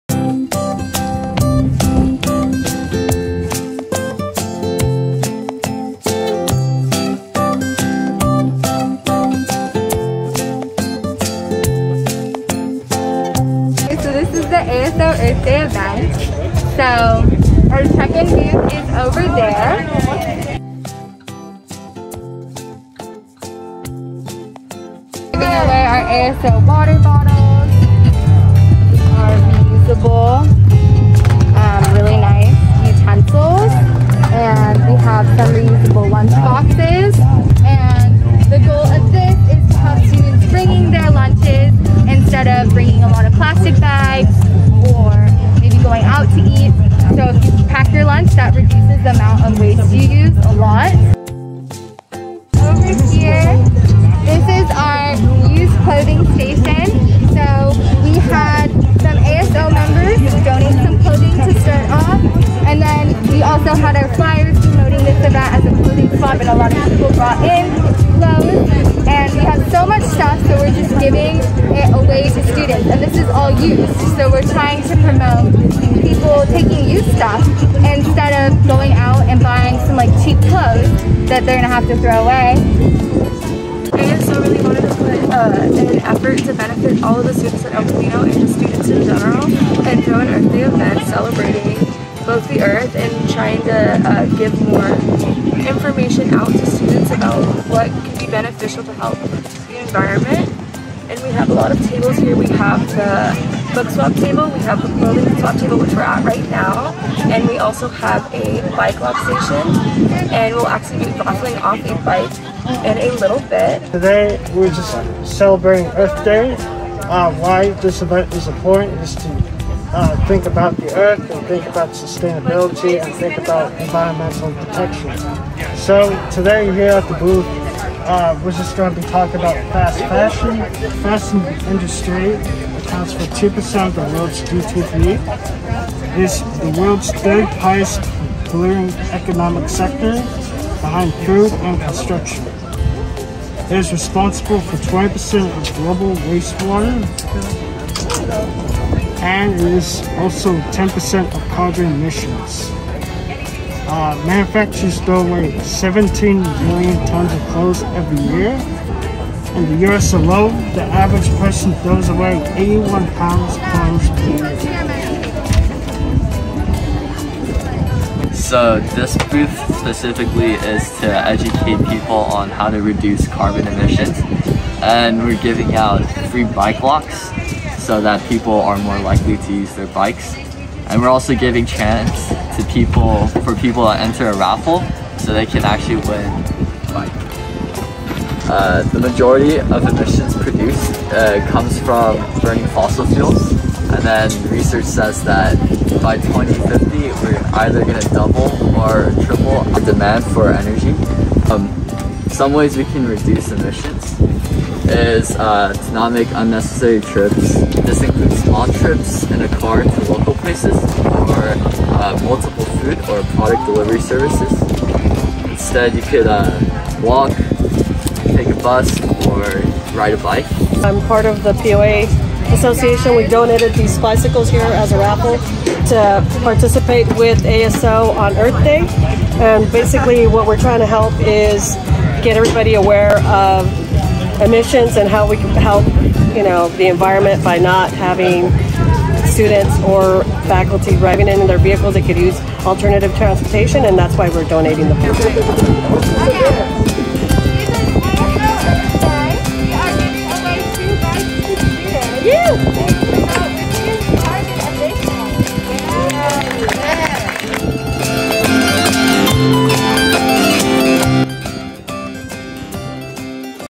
El Camino College celebrates Earth Day
The Associated Students Organization and the Environmental Club at El Camino College collaborated to host an Earth Day event on Monday, April 25 to promote sustainability. In the event, free reusable items are given out to students visiting the Student Services Plaza.